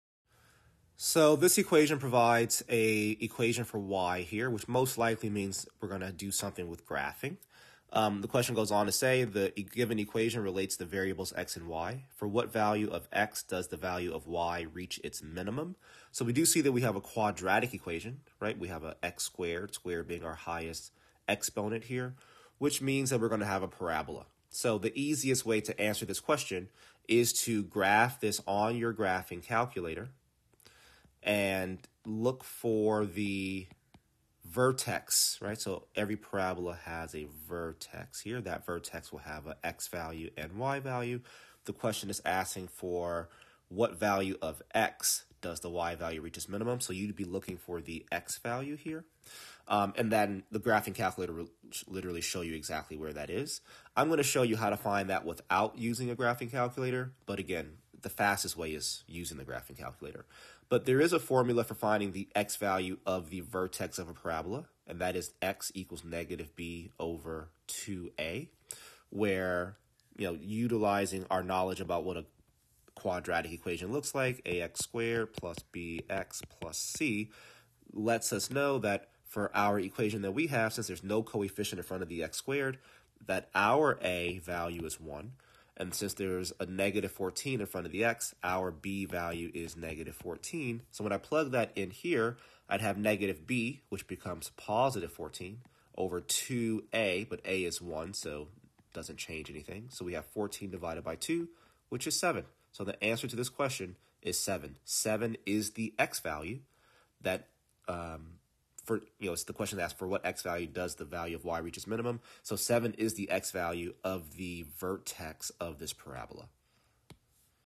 y=x^2-14x + 22; The given equation relates the variables x and y. For what value of x does the....
Nonadaptive Digital SAT Practice Test 3 Module 2 Question 7:

y=x^2 - 14x + 22

The given equation relates the variables x and y.  For what value of x does the value of y reach its minimum?